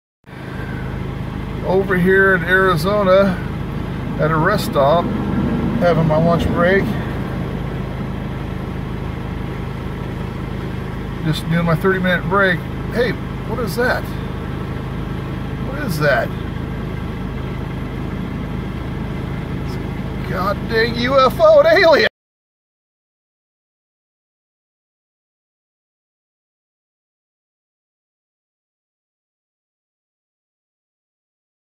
Ufo in Arizona
Ufo spotted in Arizona May 17th 2019.